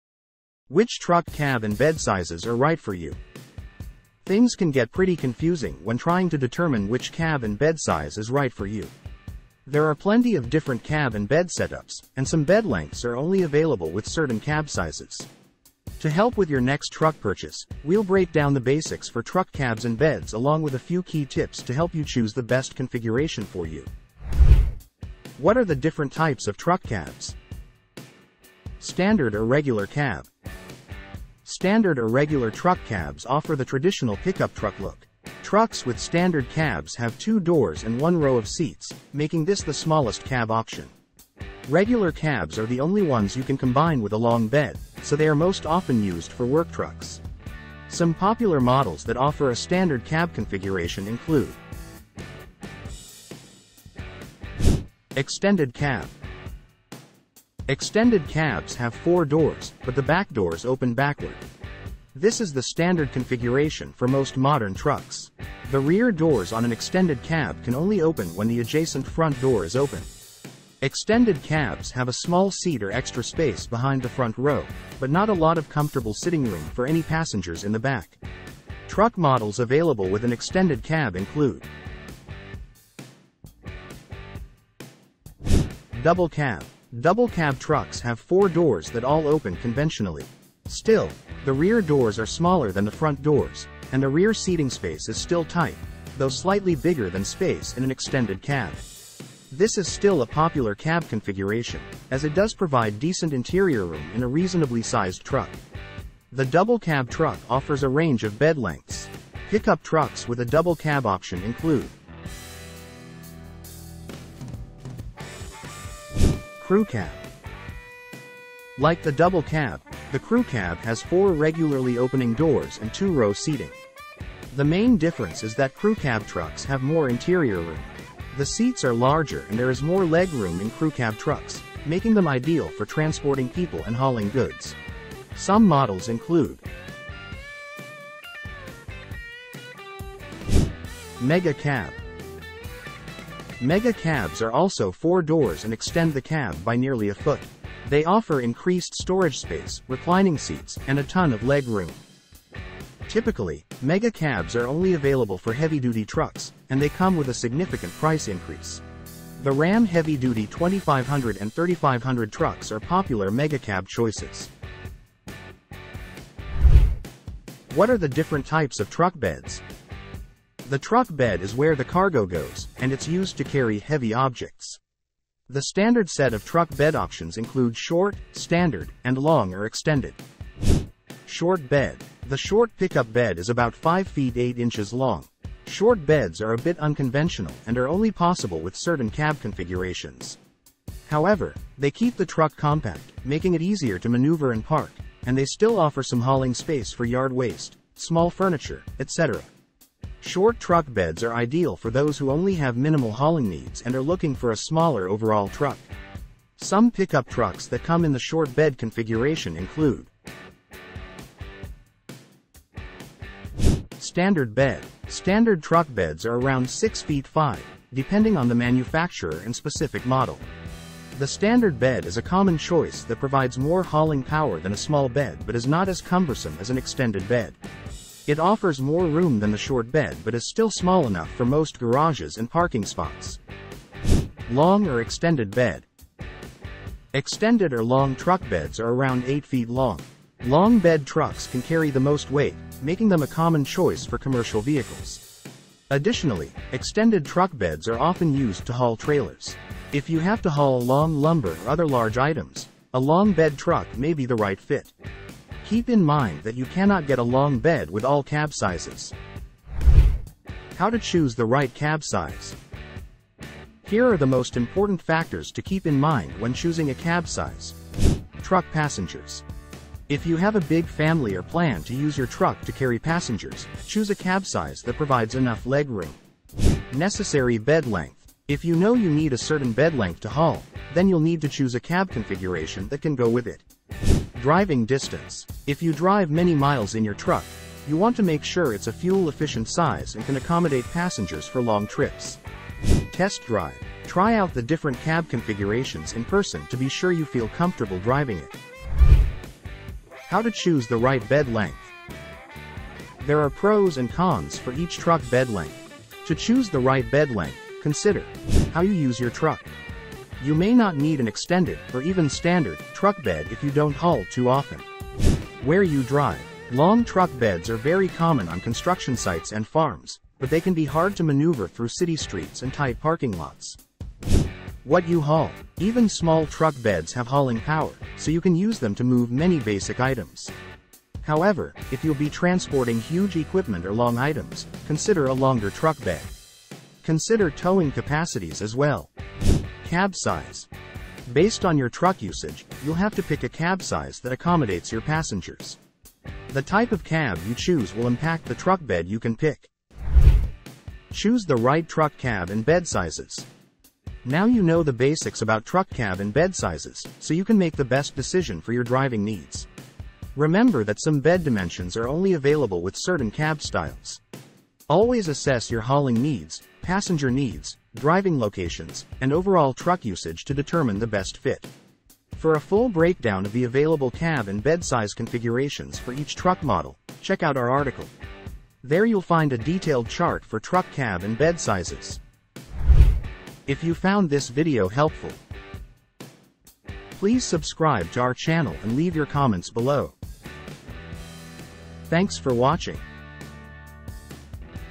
Choosing the Perfect Truck Cab & Bed Size: A Buyer’s Guide
To learn more about truck cab and bed sizes, check out

Do you have car questions and need answers now?

Video Chapters:
0:26 - What Are the Different Types of Truck Cabs?
2:47 - What Are the Different Types of Truck Beds?
4:21 - How to Choose the Right Cab Size
5:00 - How to Choose the Right Bed Length
5:51 - Choose the Right Truck Cab and Bed Sizes

In this detailed guide, we cover:

Types of Truck Cabs: From standard cabs to mega cabs, learn about each type's features and which trucks offer them. Discover the benefits of different cab sizes, whether it's the compact regular cab or the spacious mega cab.

Types of Truck Beds: We explain the pros and cons of short, standard, and long beds. Find out which bed length fits your hauling needs and how each interacts with different cab sizes.

Choosing the Right Cab Size: Consider factors like passenger space, legroom, and vehicle maneuverability to select the best cab size for your lifestyle.

Selecting the Right Bed Length: Assess your typical cargo to decide on the optimal bed length. We'll help you understand the balance between maneuverability and hauling capability.

Practical Tips for Buyers: From family considerations to test driving different models, we provide actionable advice to ensure you pick the perfect truck for your needs.

By the end of this video, you'll have a clear understanding of how to match truck cab and bed sizes to your specific requirements, ensuring you make a choice that offers both functionality and comfort.